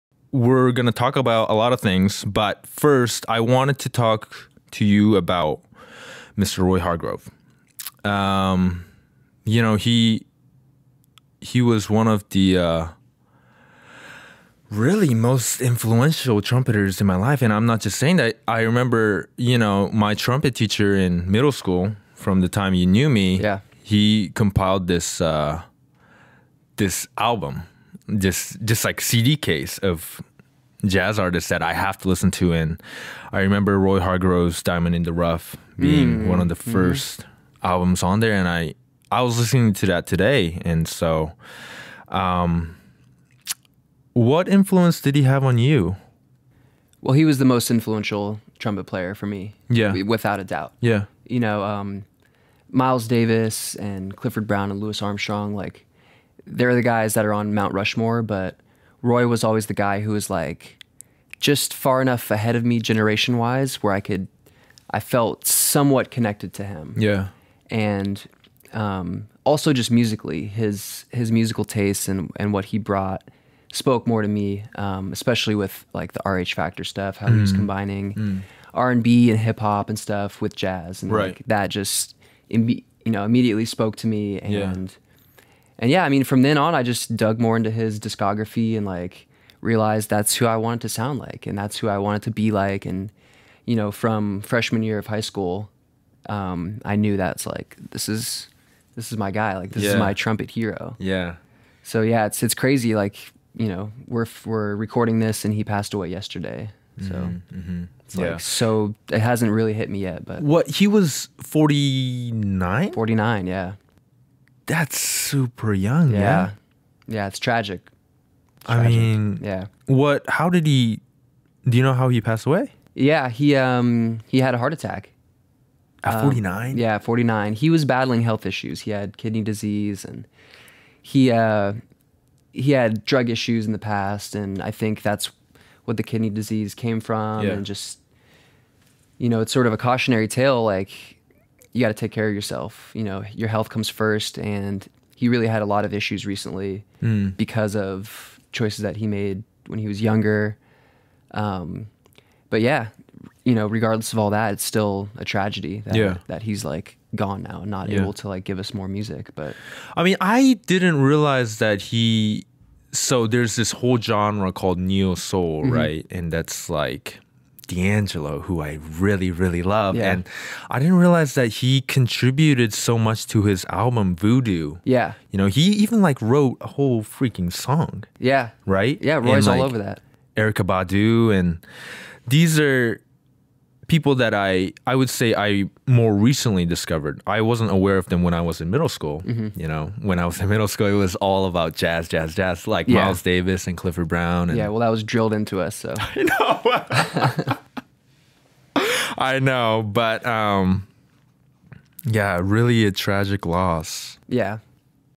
JJ Kirkpatrick Pays Tribute to Roy Hargrove
JJ Kirkpatrick talks to Kai about Roy Hargrove and what an influence he was on him.

Filmed by Cheryl Ellzysmith.